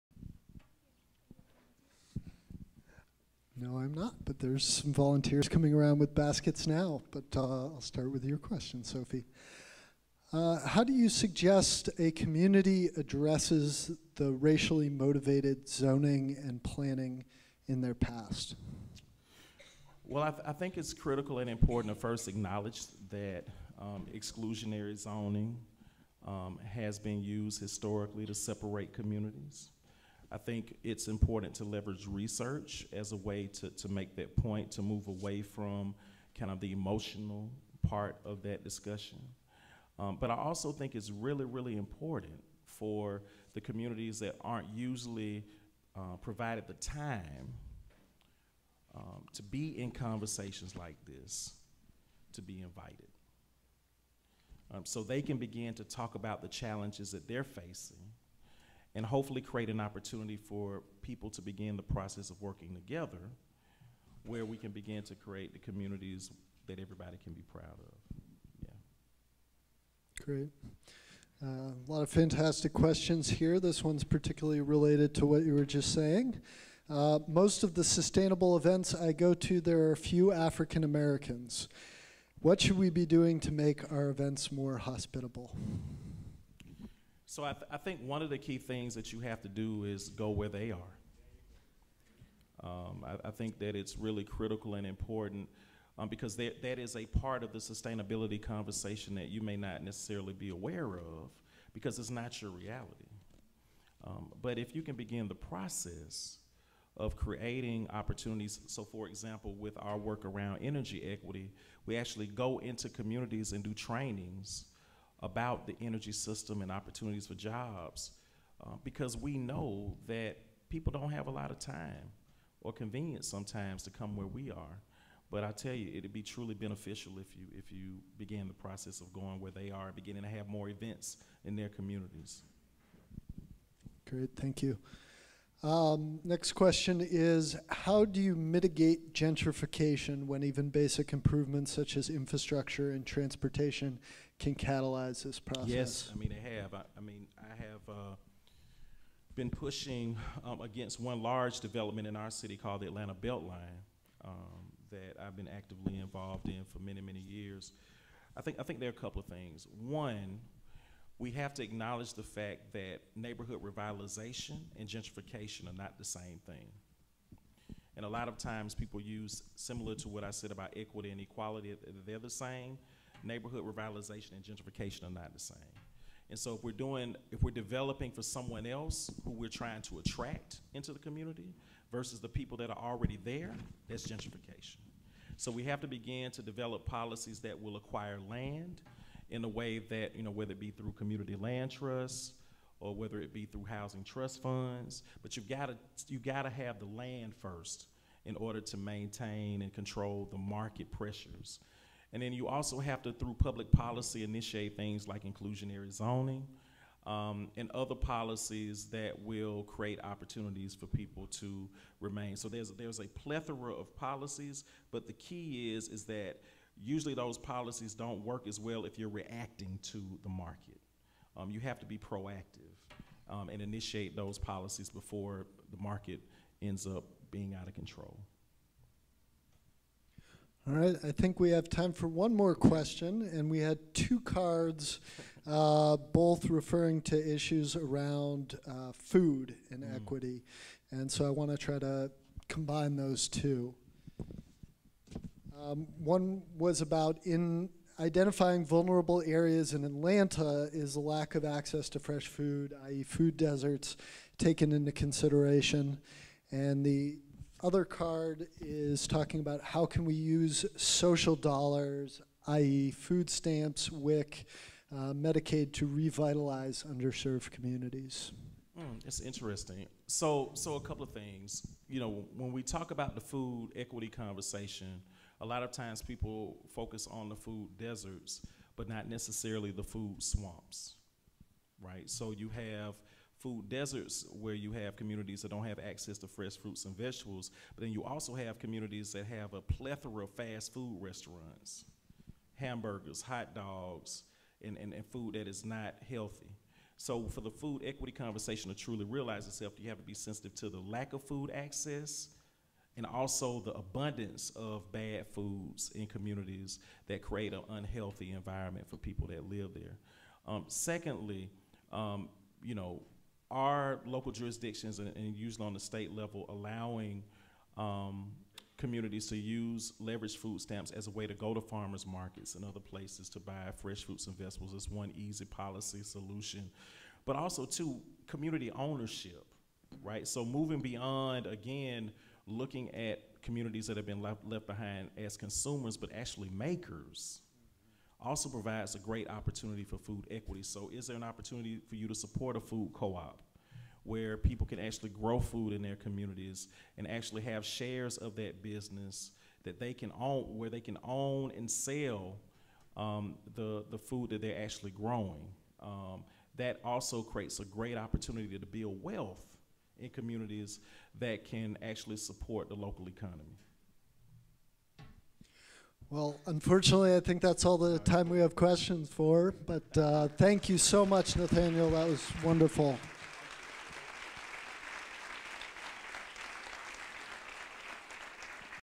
2017 Sustainable Communities Workshop – Q-and-A with Nathaniel Smith
Nathaniel Smith, founder and chief equity officer
(CEqO)/CEO of the Atlanta-based Partnership for Southern Equity (PSE), fields audience questions about social equity.